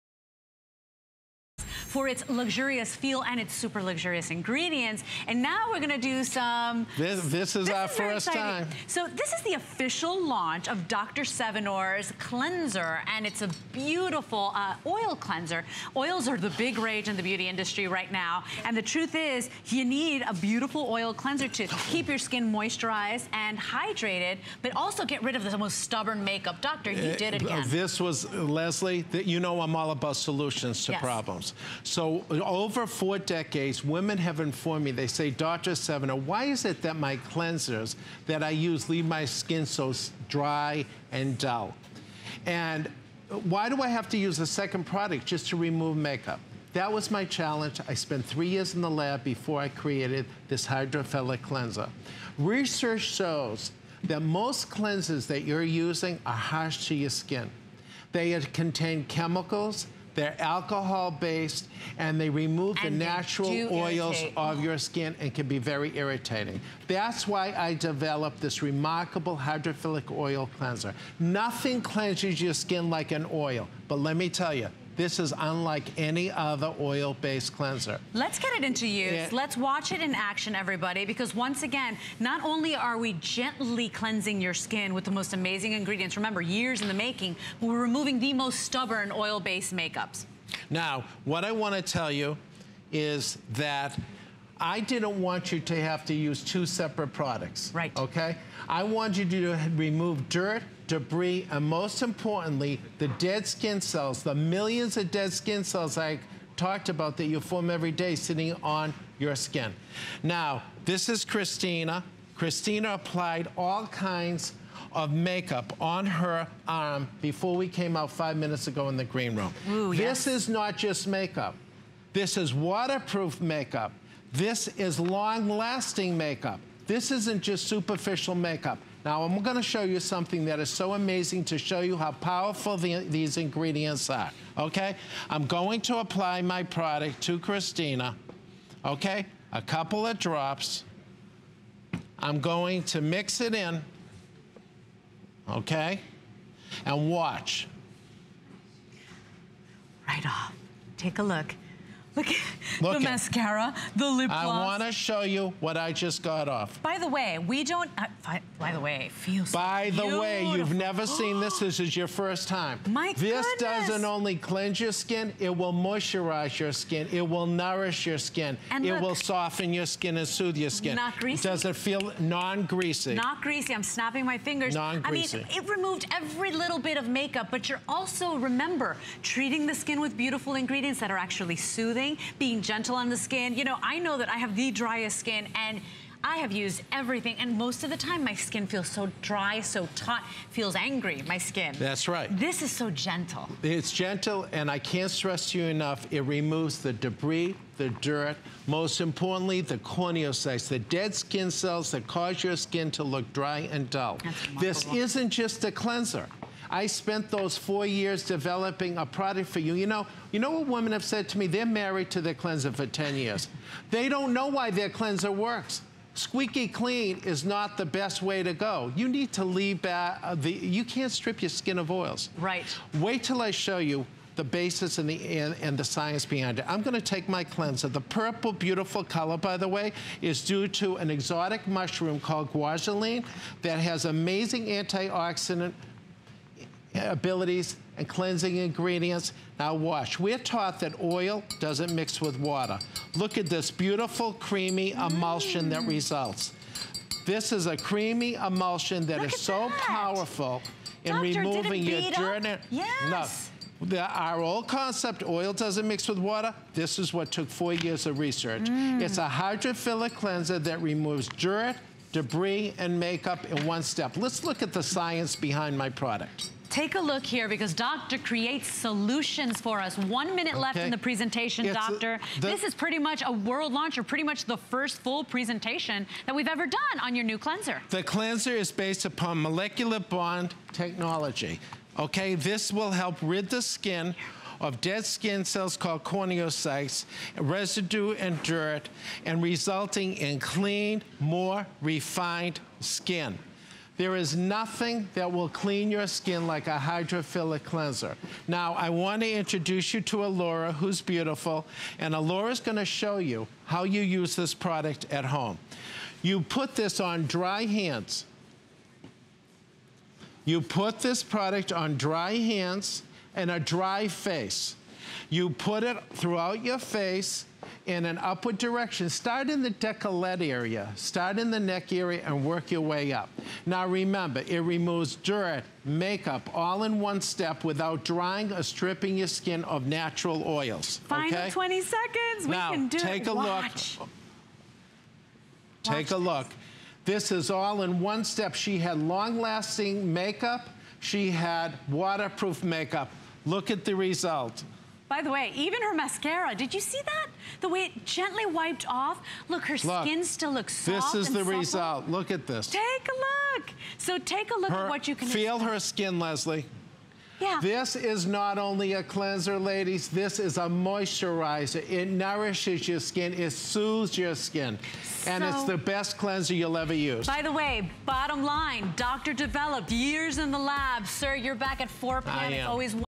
Dr. Sevinor Cleansing Solution Hydrophylic Oil
Clean skin is the first step to gorgeous skin. Made with sweet almond and argan oils, this facial cleanser helps remove dirt, oil and makeup,...